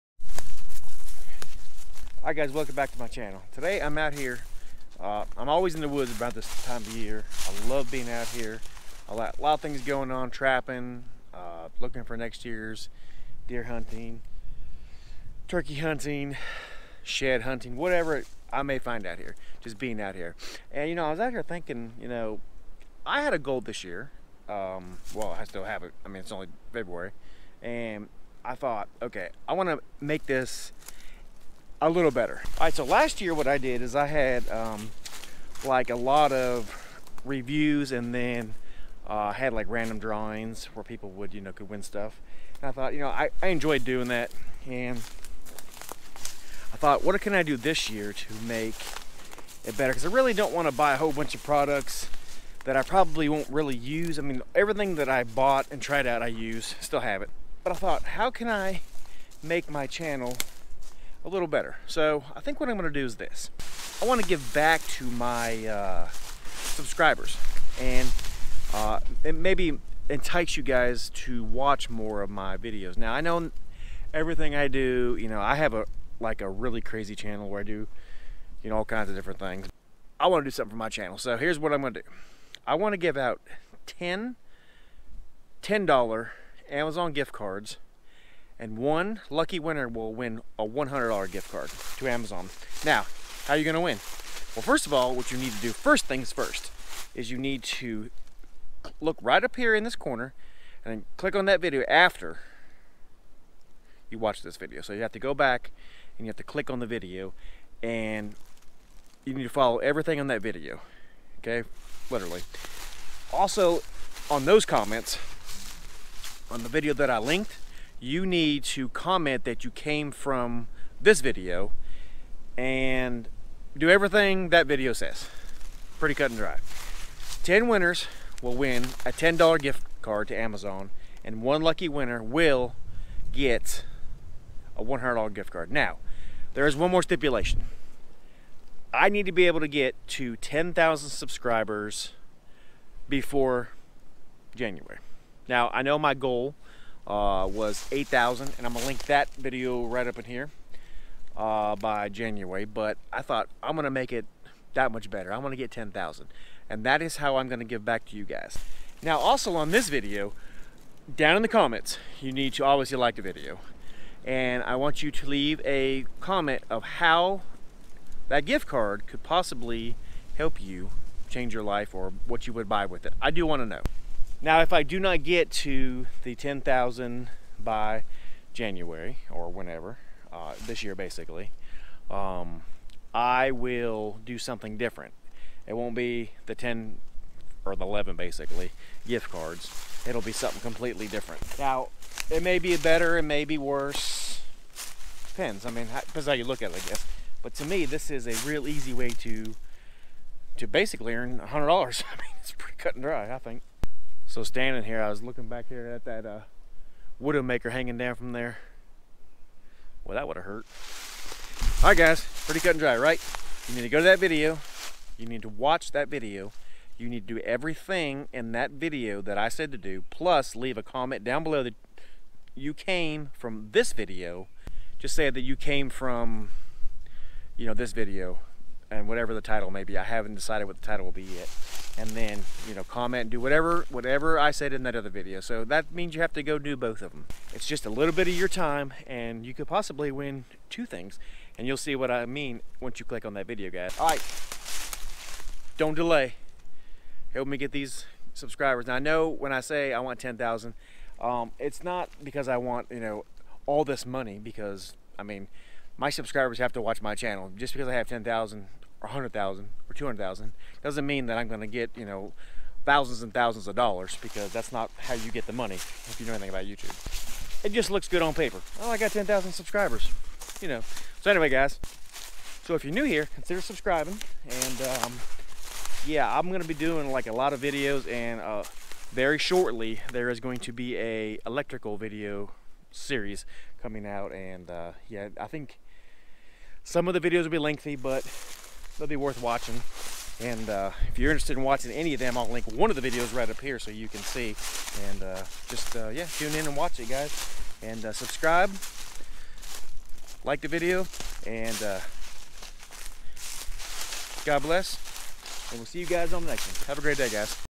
Do You Want To Have Free Amazon Gift Cards??? Yes You Do
Do You Want To Have Free Amazon Gift Cards??? Yes You Do.  Here is the easiest thing you will every do to win Free Yes FREE Amazon Gift Cards.  I will be giving out 10 Ten Dollar Gift Cards and one luck person will win a 100 dollar free amazon gift card. Now how do you do this?? First of all you need to watch this whole video all the way through. then go back click on the video I tell you to click on. On that said video comment that you came to that video from the free amazon gift card video. And comment on that video also if you want a chance to win that give away as well...then come back here comment and like this video..And that is it. pretty easy so what are you waiting for??